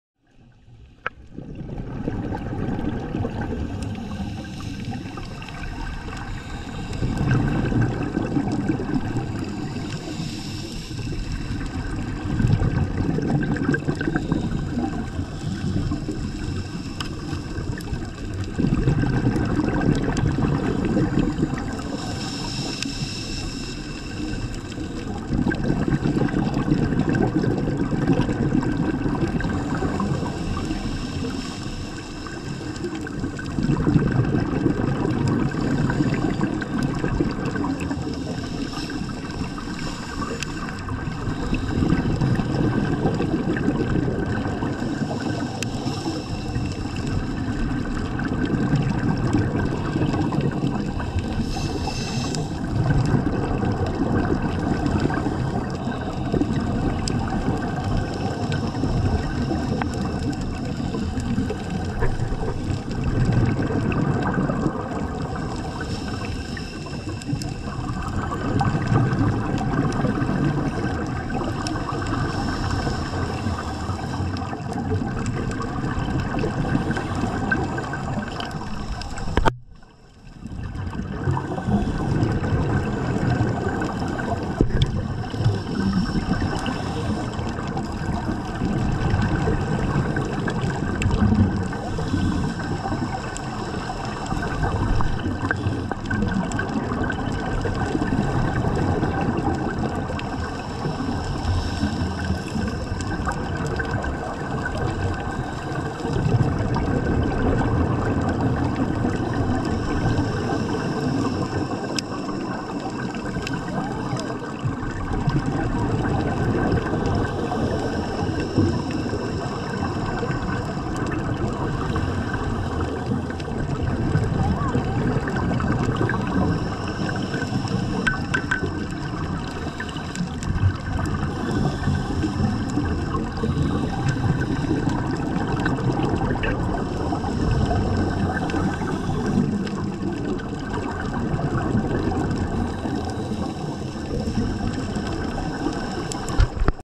Curacao - Octopus
Octopus spotted while night diving in Curacao